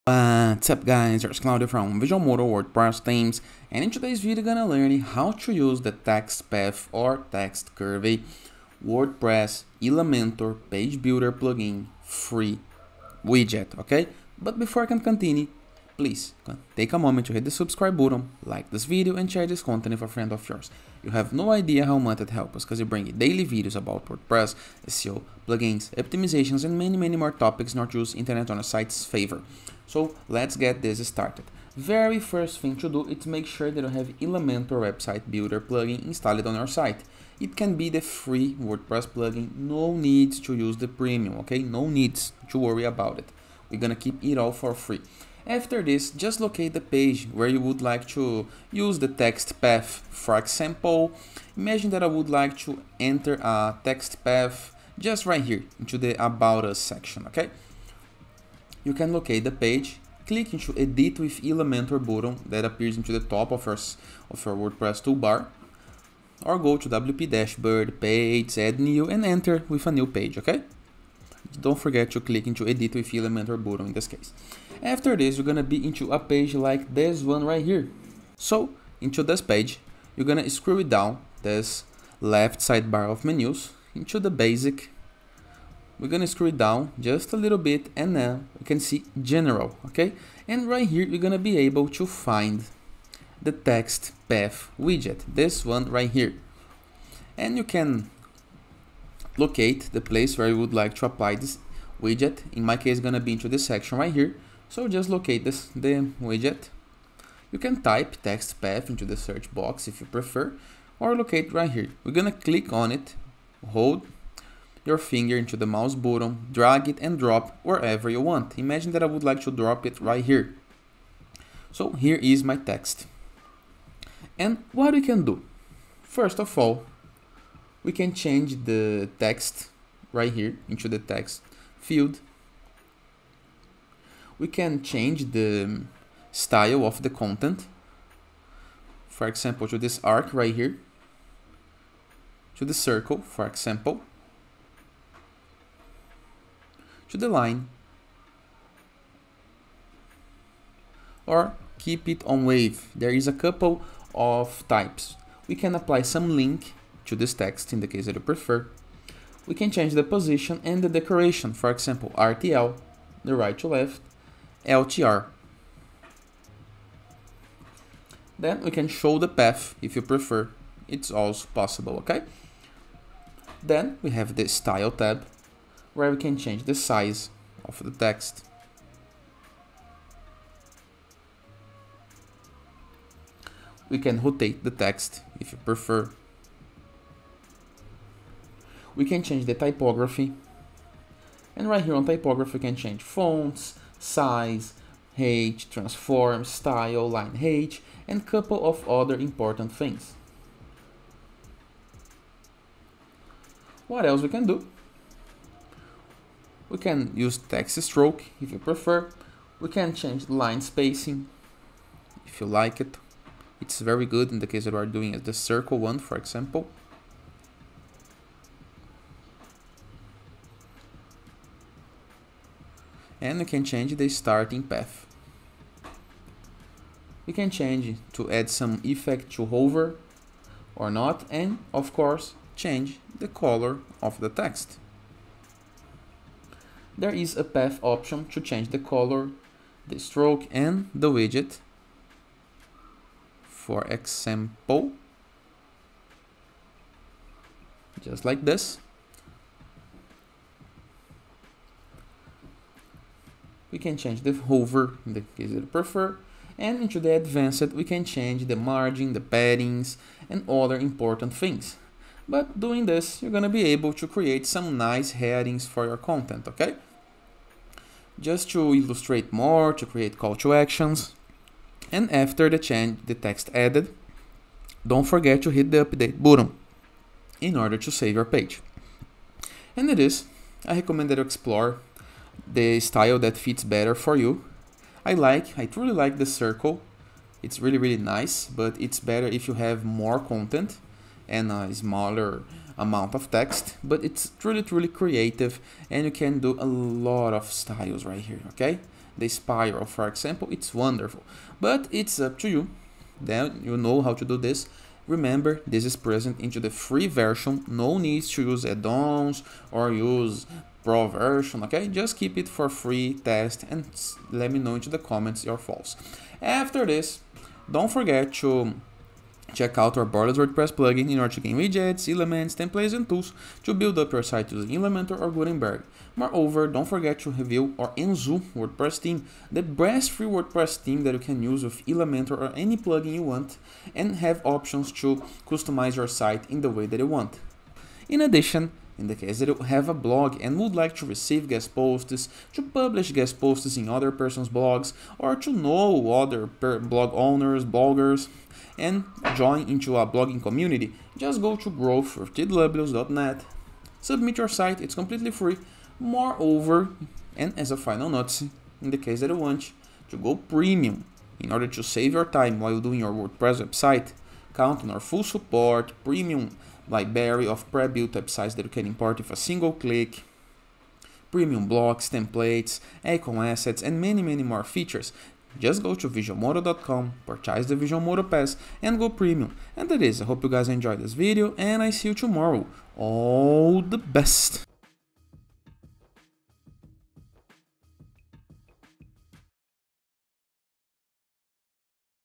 How To Add or Use Text Path & Curvy in Elementor Free WordPress Plugin? Widget Tutorial
In this tutorial video, we will guide you step-by-step on how to add or use Text Path & Curvy features in Elementor, a free WordPress plugin. This video is perfect for beginners who want to enhance their website design skills.

We will cover everything from installing the plugin, navigating through its interface, to creating stunning text paths and curves. By the end of this video, you'll be able to create dynamic and visually appealing designs on your WordPress site using Elementor.

By utilizing the Text Path Widget, you can imbue your text with any shape! select from a predefined list of shapes or create your own SVG shape, alter and style it as desired.